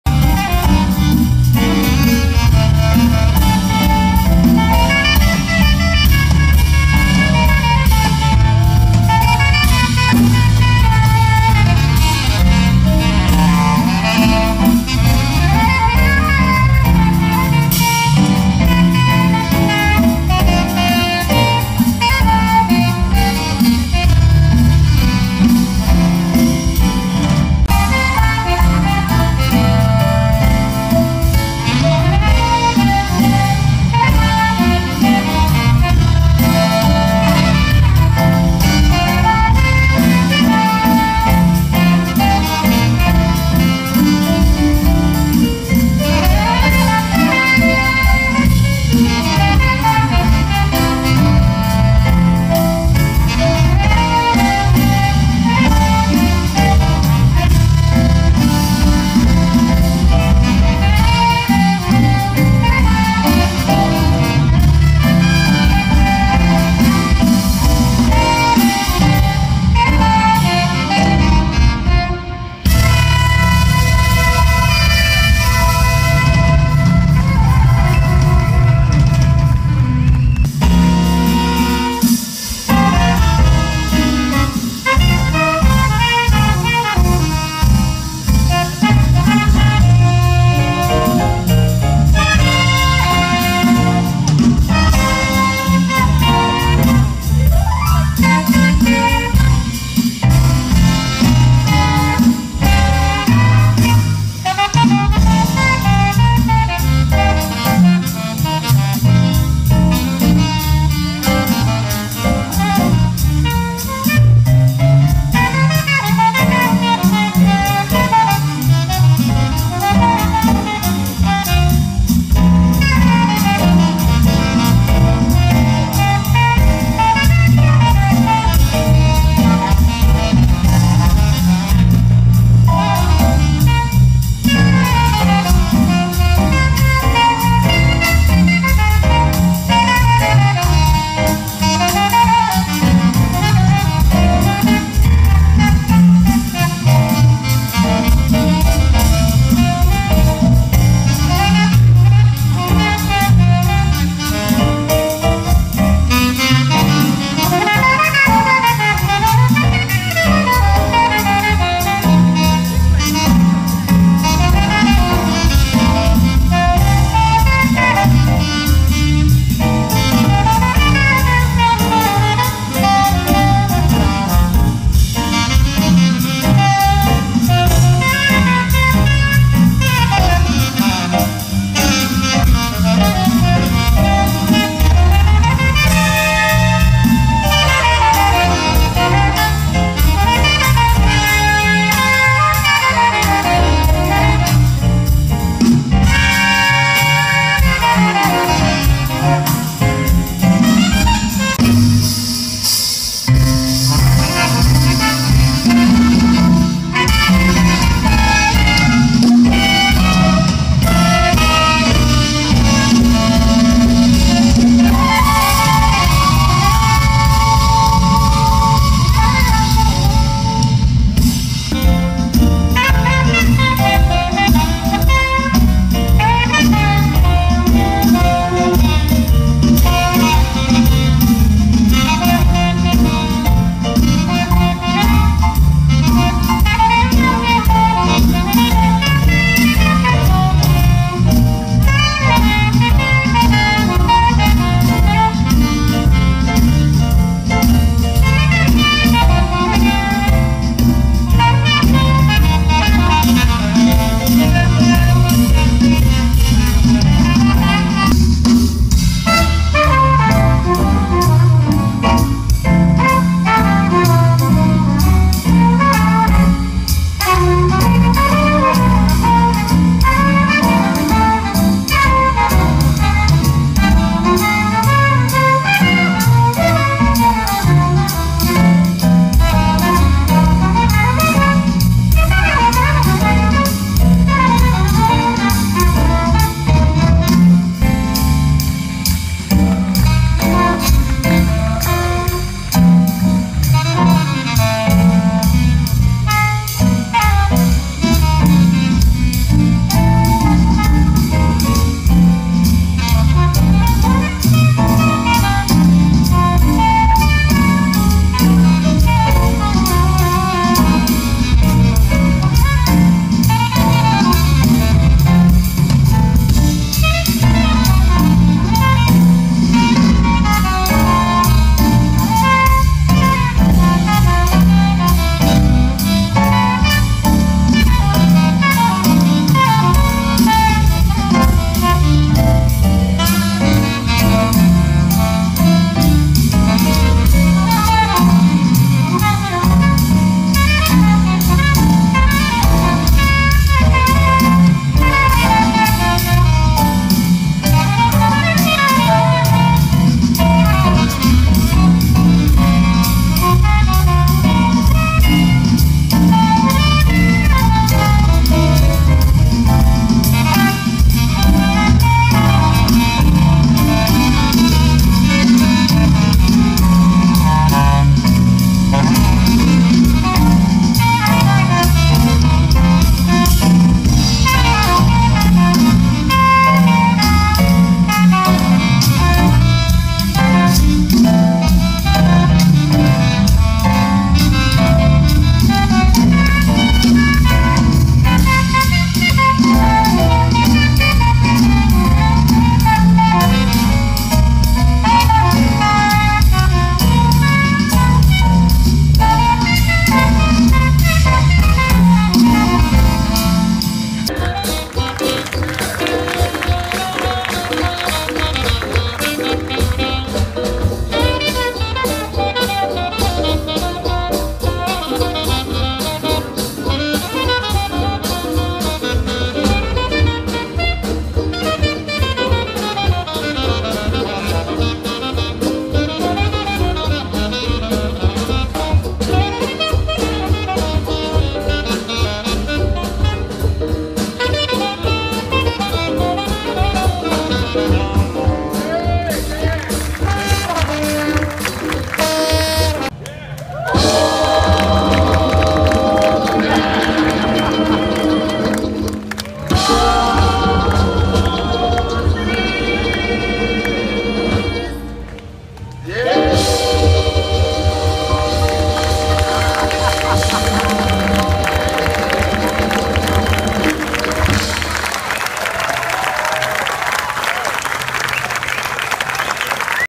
Kasan Belgrave Performance July & August 2014 (Audition Video 3)
Paradise Valley Jazz Festival & Detroit Jazz Festival